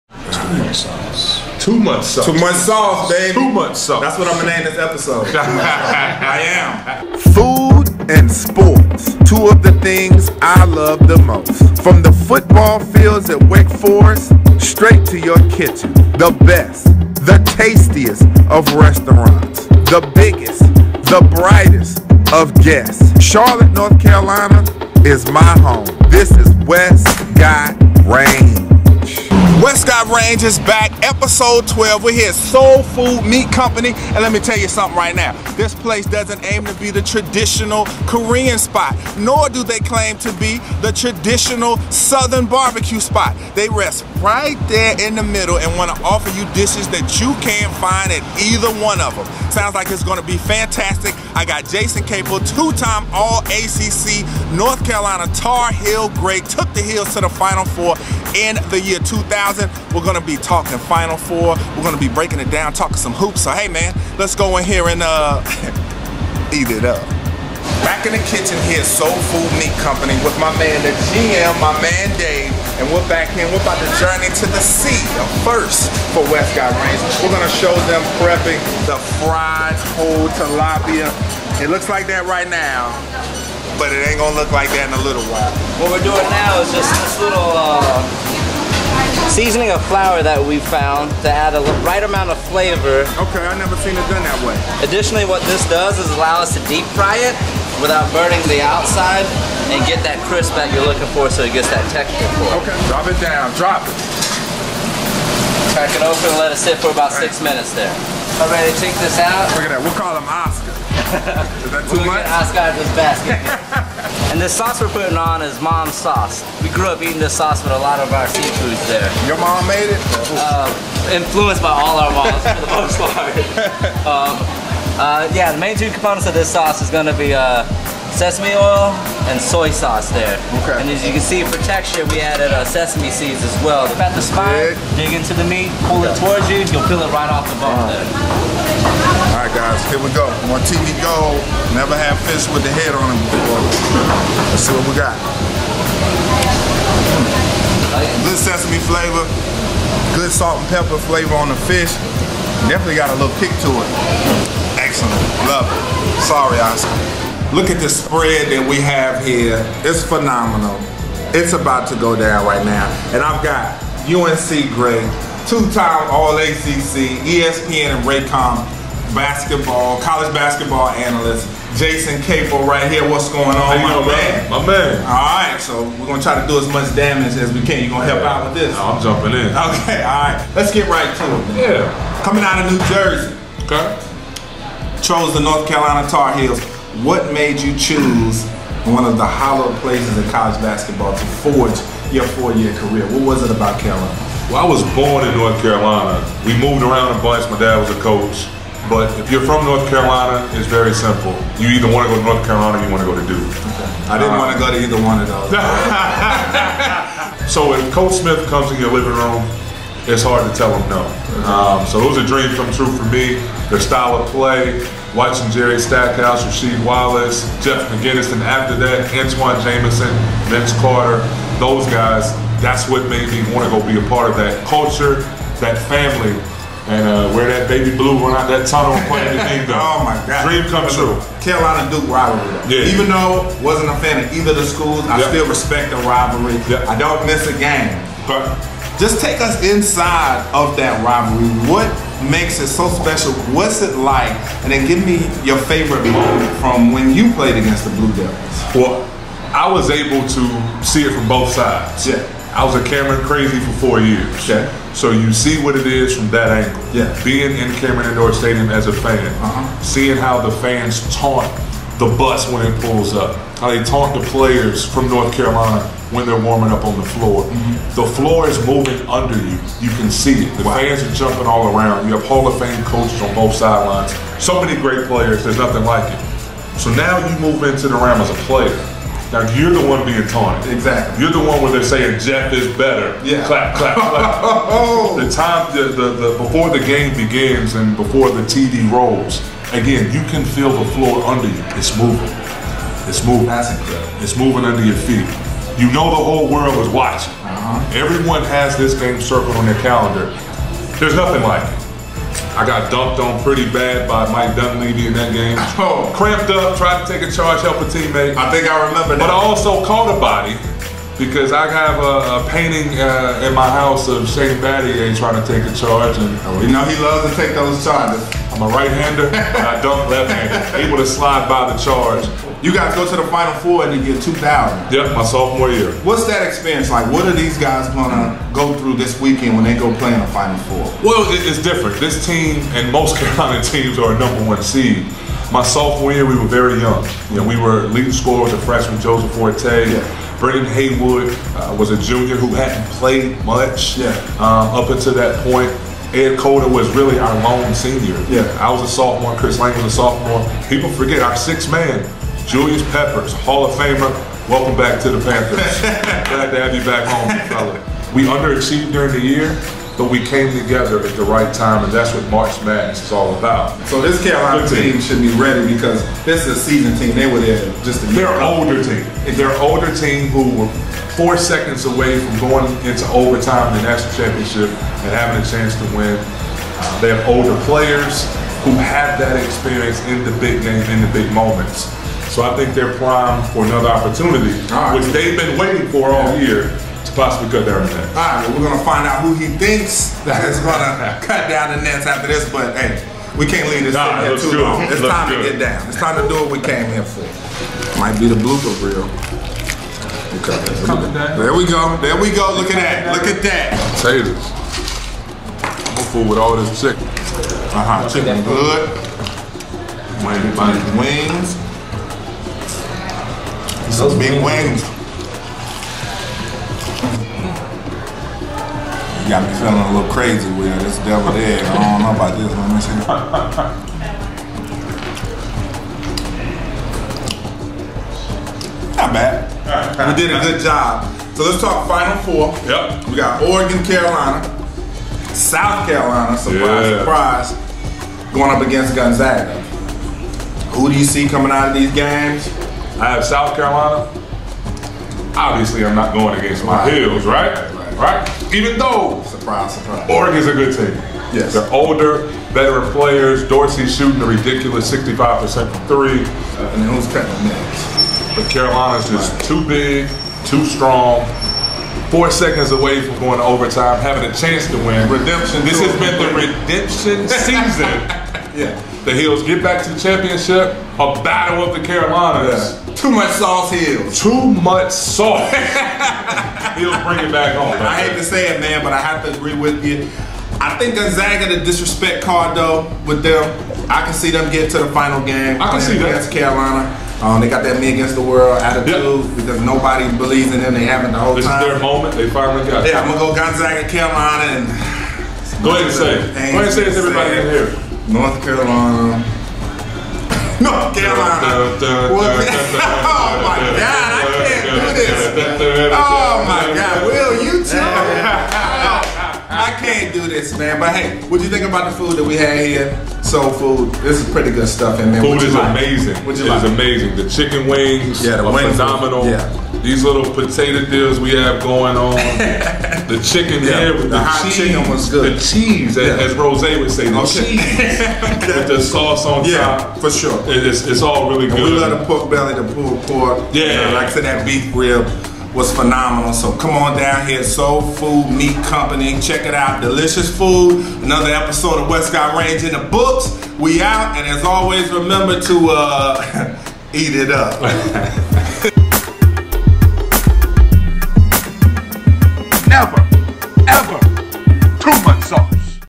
Wes Got Range: UNC Basketball Alum Jason Capel Interview
Episode 12: We check out SEOUL Meat Company in Charlotte, NC to show them how they prep some of their signature dishes and discuss Jason Capel's Final Four run with UNC!